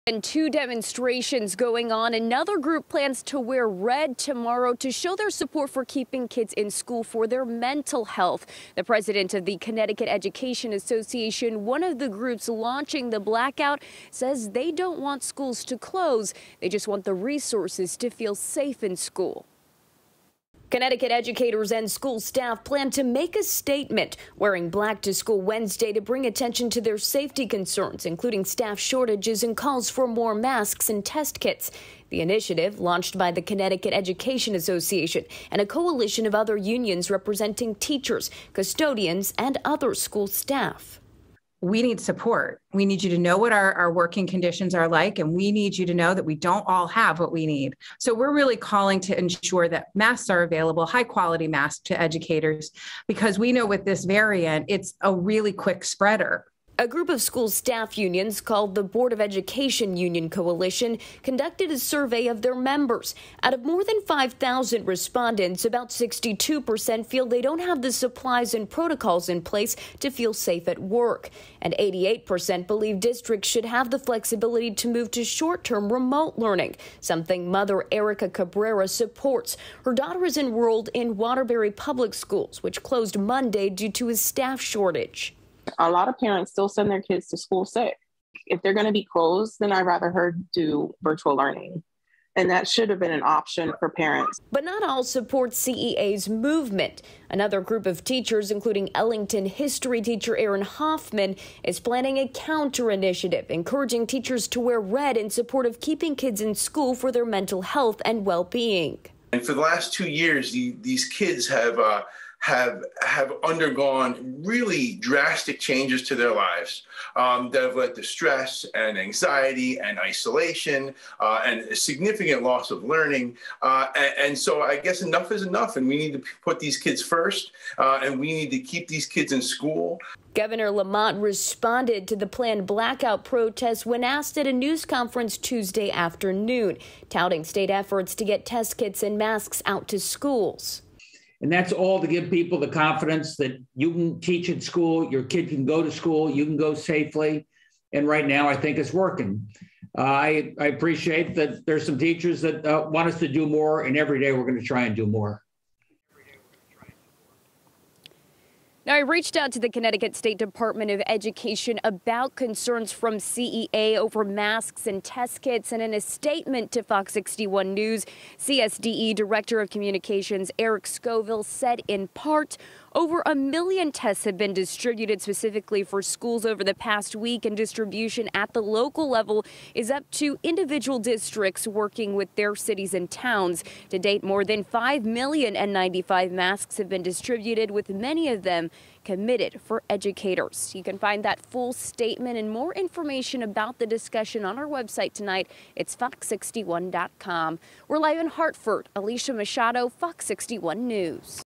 CT educators, school staff launch initiative to ensure schools are COVID safe
Connecticut educators and school staff plan to make a statement, wearing black to school to bring attention to their safety concerns.